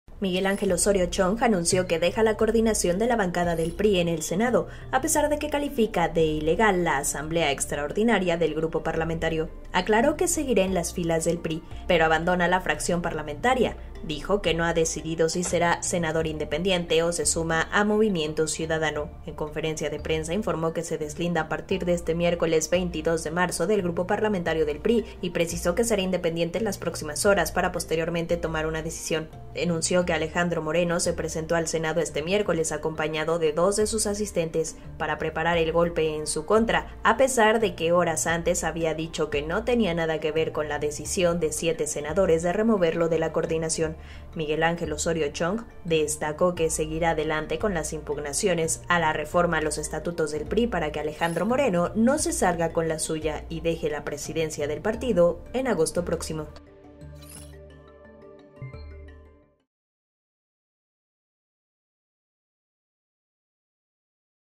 Anuncia Osorio Chong su salida de bancada del PRI en el Senado
"Voy a seguir con las impugnaciones, voy a seguir con el PRI" advierte Osorio Chong